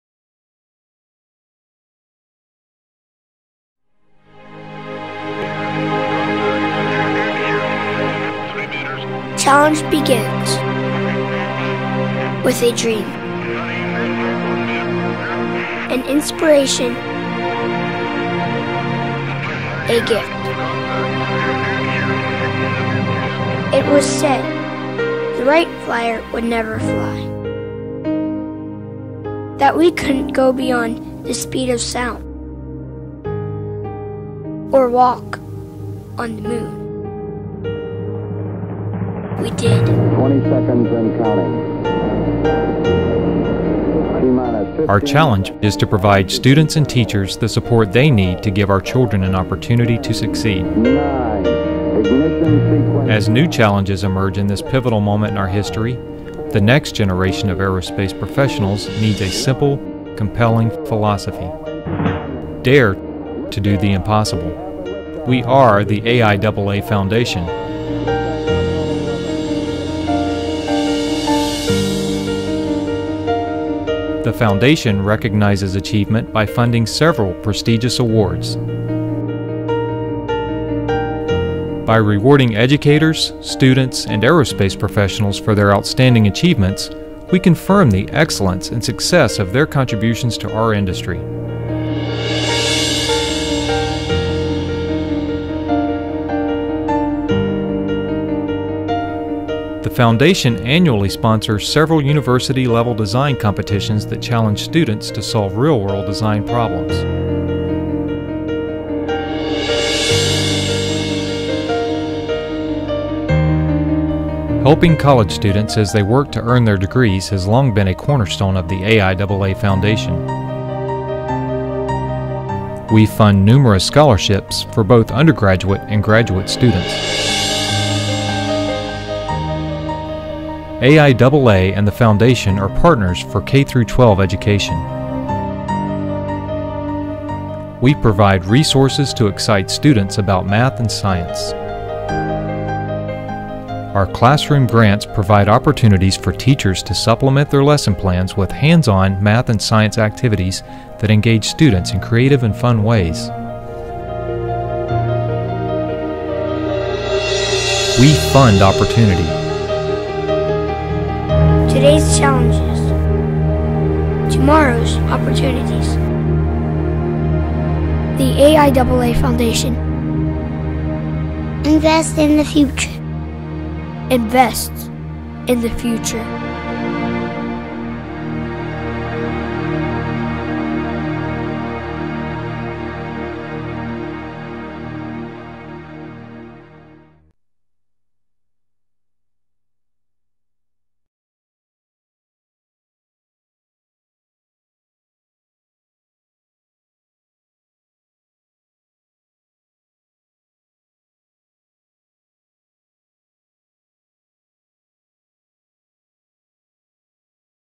AIAA Foundation Video.mp4
Did you know...that the AIAA Foundation offers professional awards, scholarships, STEM education grants and more? Check out the AIAA Foundation video to discover the amazing opportunities the AIAA Foundation provides, and how you can Invest in the Future of Aerospace!